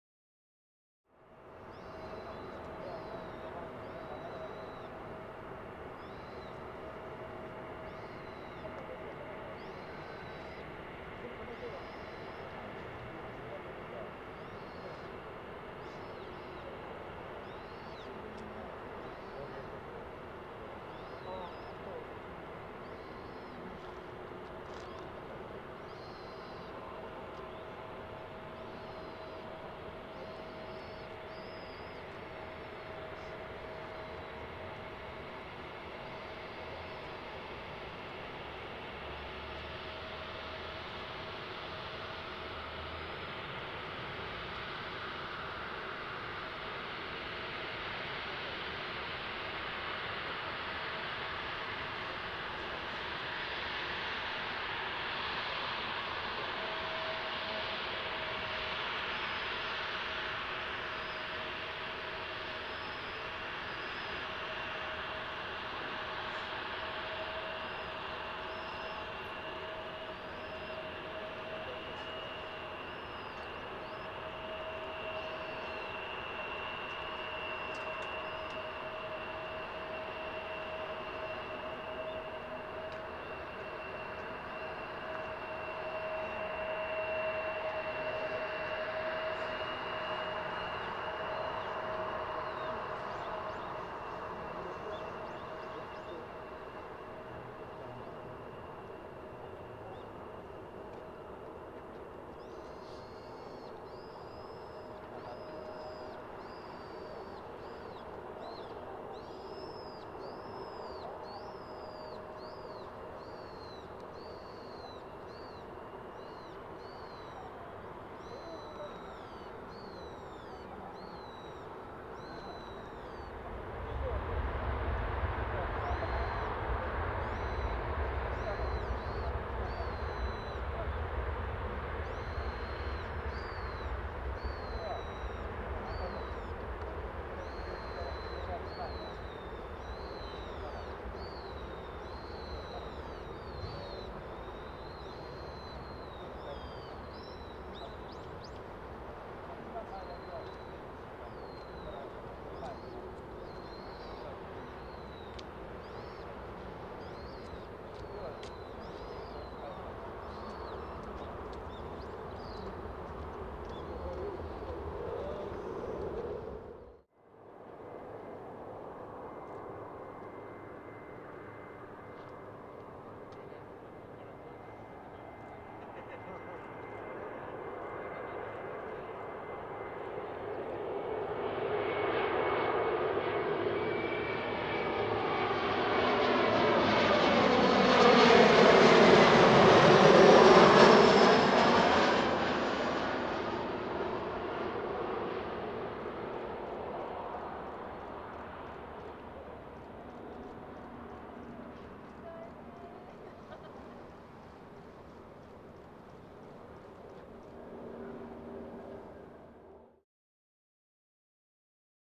[4K] FDA ERJ-175(JA16FJ) - 県営名古屋空港 2020年12月11日
2020年12月11日に県営名古屋空港（小牧基地）のR/WエンドでHC-X1000を使用し撮影した『FDA ERJ-175(JA16FJ)』の4K動画です。